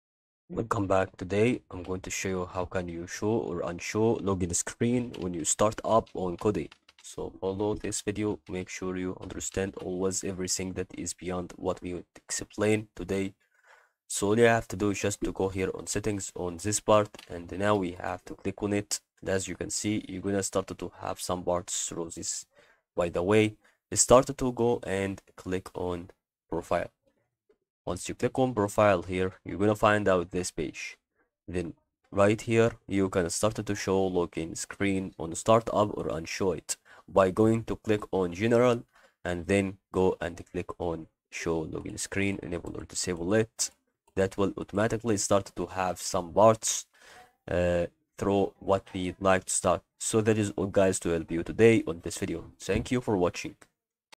How to Show or Unshow Login Screen When Startup on Kodi
Customize your Kodi experience by learning how to show or hide the login screen on startup with this comprehensive guide. Whether you prefer quick access to your media or require authentication for security purposes, configuring the login screen behavior is essential. Follow these step-by-step instructions to adjust the login screen settings in Kodi according to your preferences.